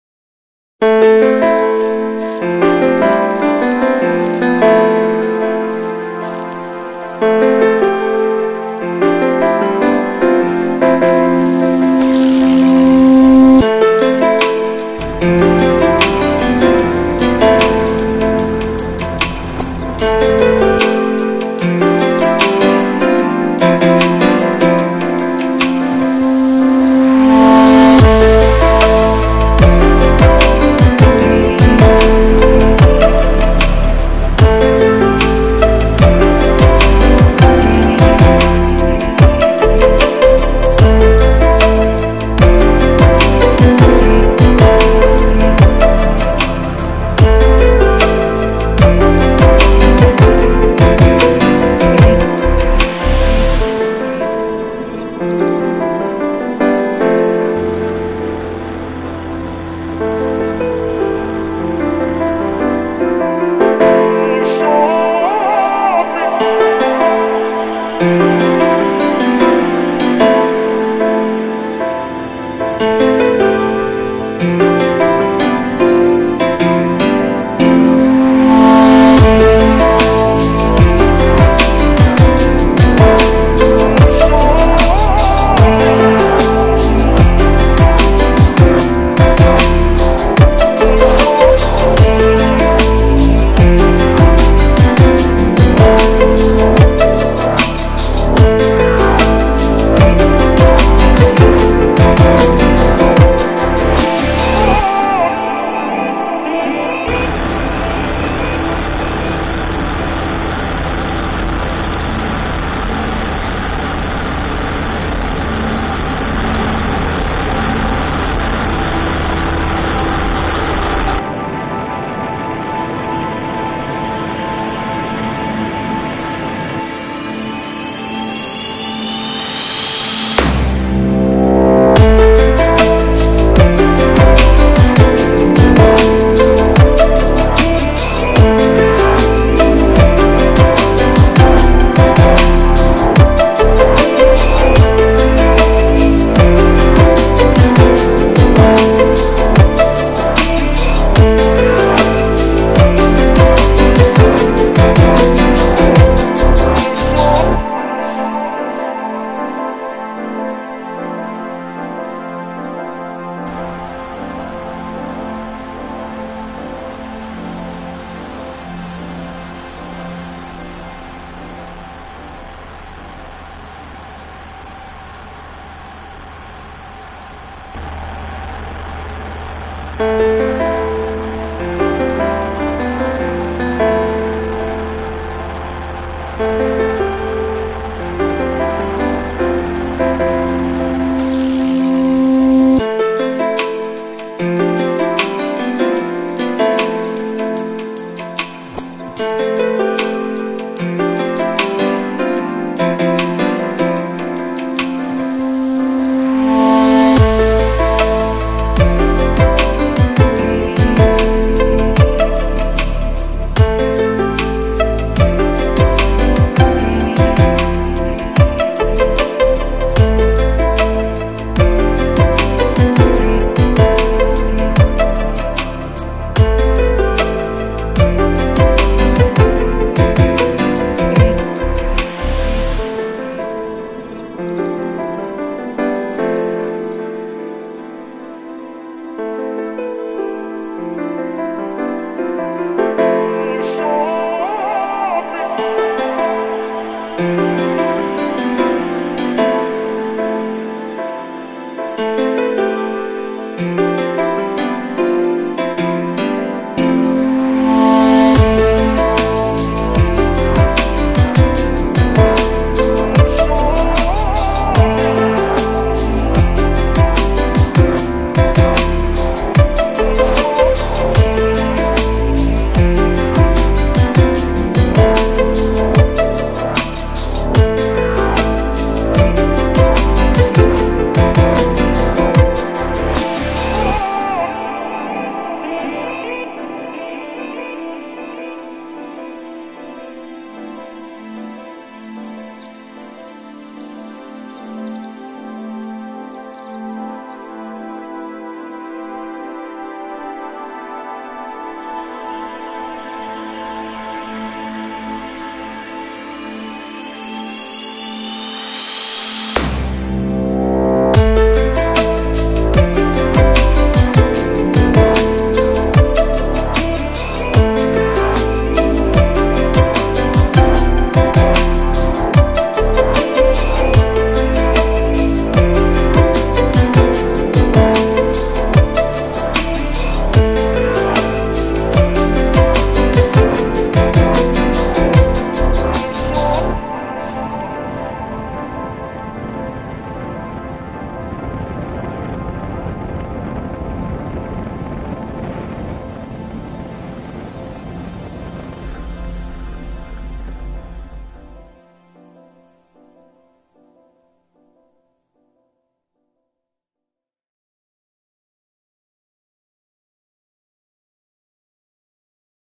FLY WITH TILLY
A remake of some videos of Tilly (classic C182A), owned by Moose, my Border Collie and, loaned to me to fly around.